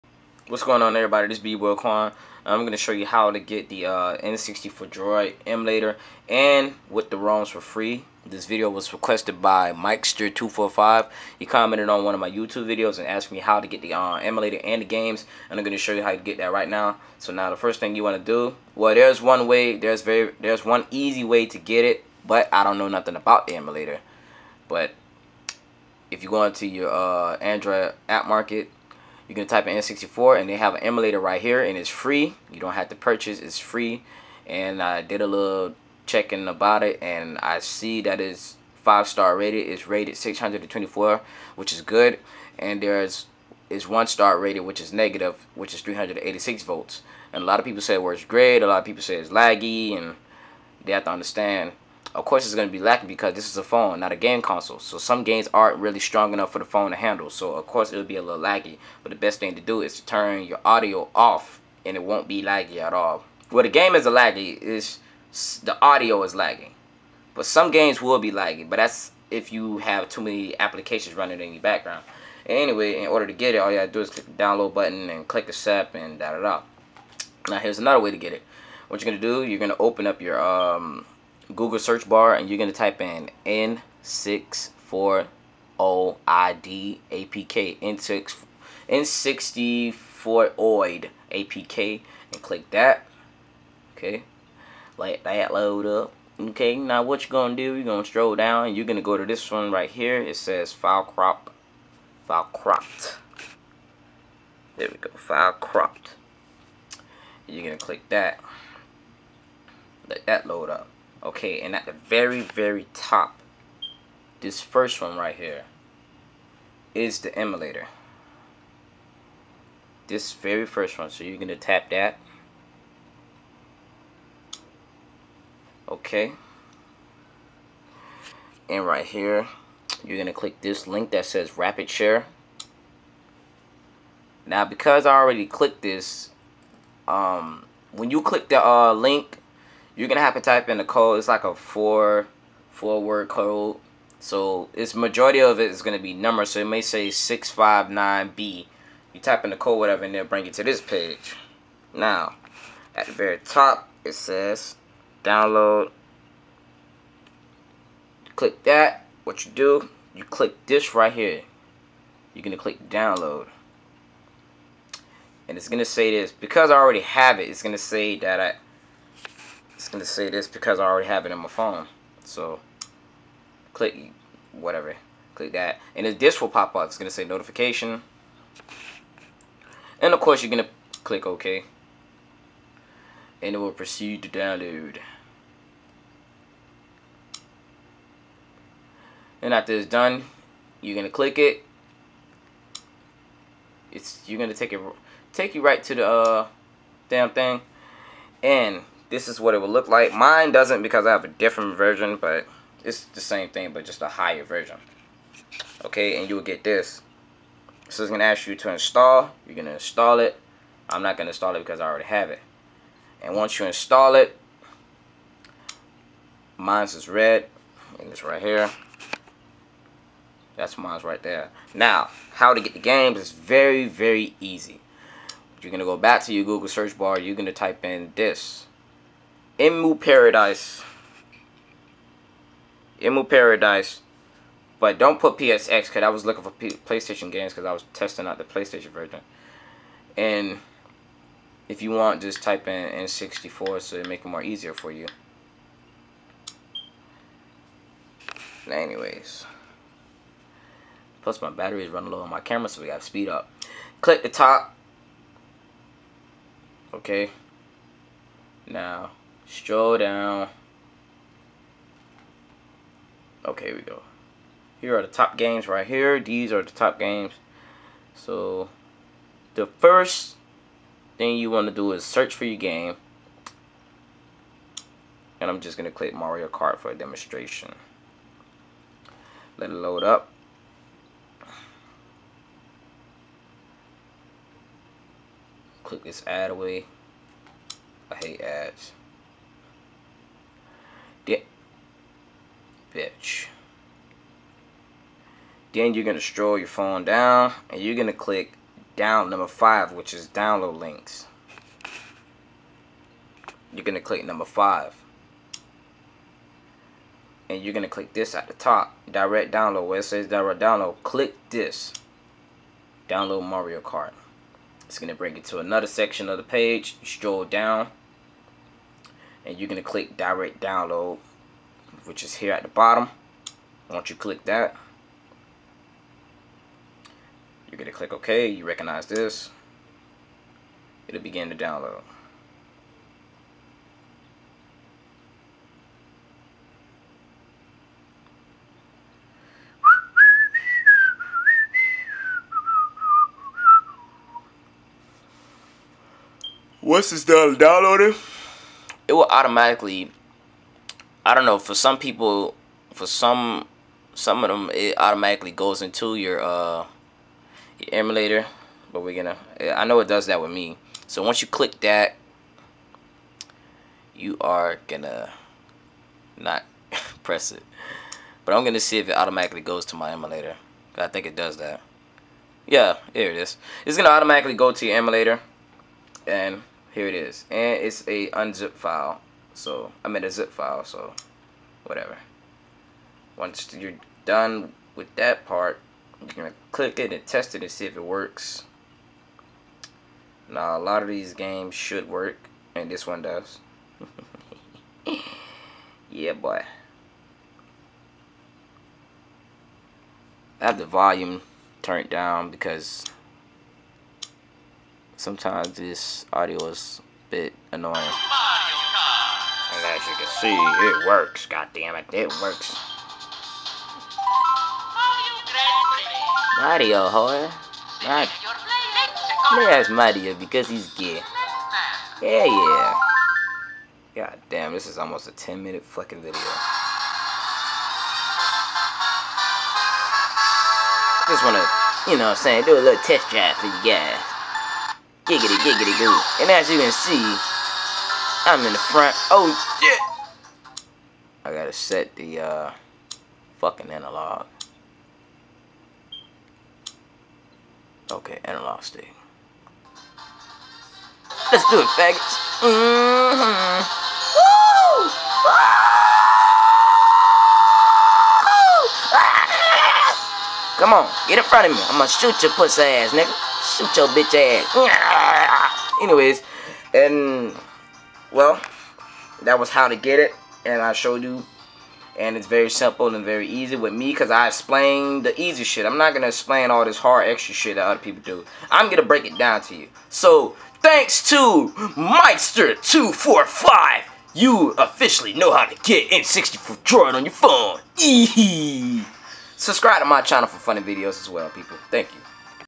How To Get N64OID & Roms Free All Android Devices
Requested by Mikester245 Shout to Him! IN THIS VIDEO I WILL BE SHOWING YOU HOW TO GET N64OID & ROMS FOR FREE ON ANY ANDROID DEVICE THATS CAPABLE OF HAVING IT REMEMBER THIS ONLY FOR THE MORE ADVANCED ANDROIDS! ALSO THERES A FREE N64DROID APP IN THE MARKET AND ITS FREE SO FOLLOW EVERY STEP YOU SEE ME DO!